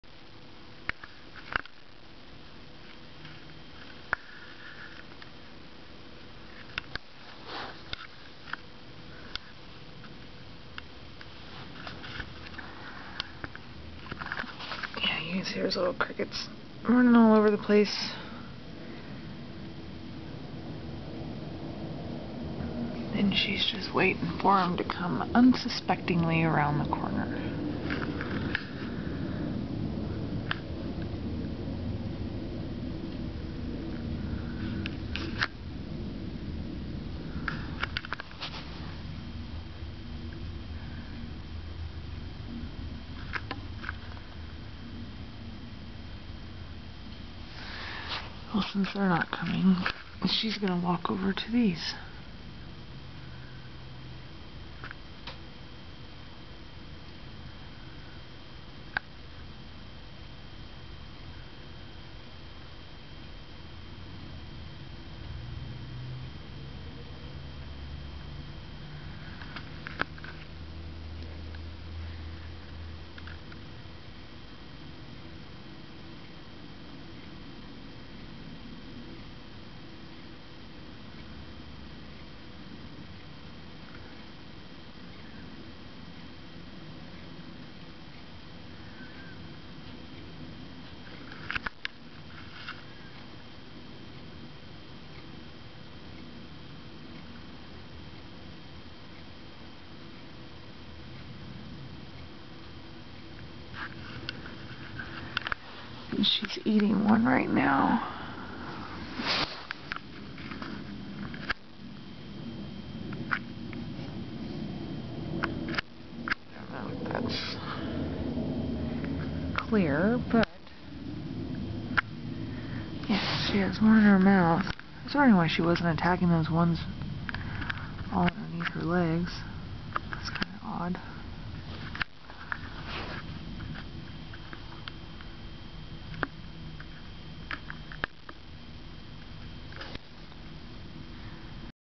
Phobia's Snack - Tarantula getting her NOM on.
My tarantula, Phobia in her tank with come crickets. She usually attacks more, ahh, viciously, but since she has gotten comfortable she kind of takes her time. Crazy redheads, just like to savor the kill. :D